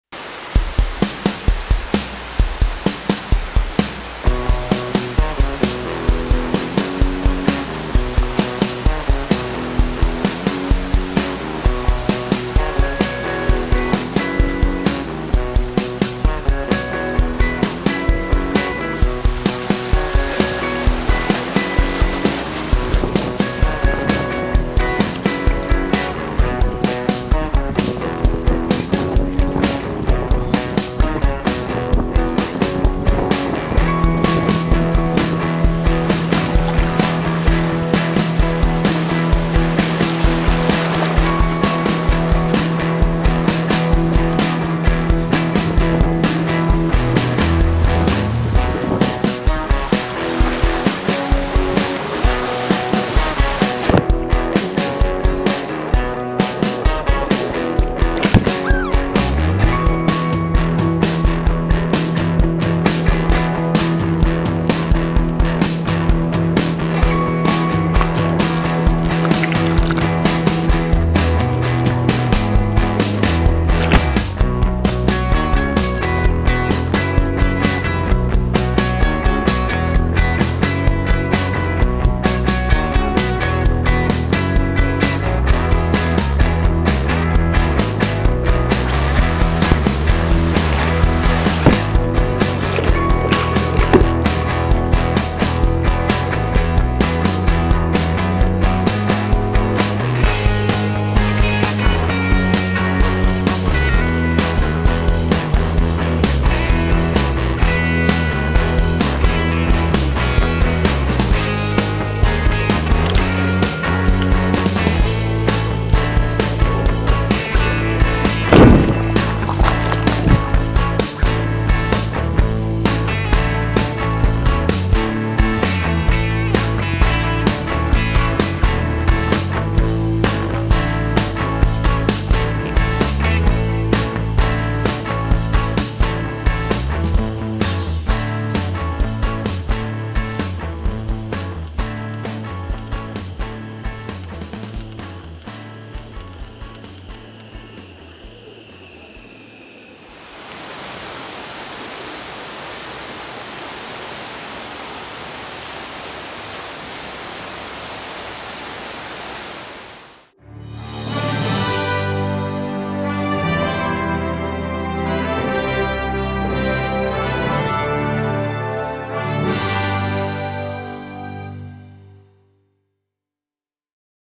Wildwasserbahn im Aqua Mundo - Center Parcs de Kempervennen
Die Wildwasserbahn im Aqua Mundo des Center Parcs De Kempervennen. Als Event findet hier zu bestimmten Zeiten auch Wildwasserrafting statt. Eine schöne Wildwasserbahn außen, welche mit zwei aufeinanderfolgenden Röhren zunächst in eine Grotte führt und anschließend auf den innenliegenden Wildwasserabhang. Auch für Kinder unter Begleitung eines Erwachsenen möglich.